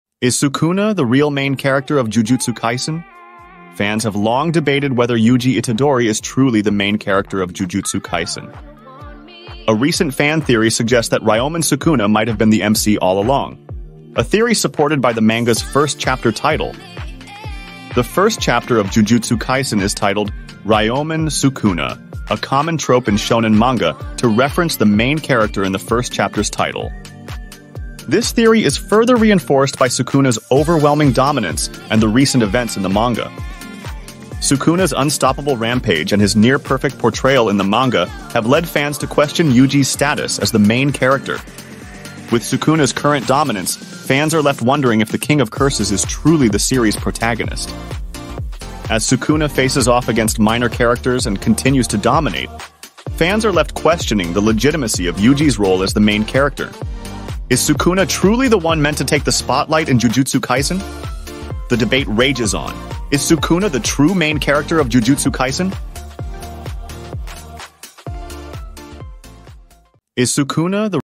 Jujutsu Kaisen MC Has Always Been Sukuna, Not Yuji (& Chapter 1 Proves It)
► Title: - Jujutsu Kaisen MC Has Always Been Sukuna, Not Yuji (& Chapter 1 Proves It)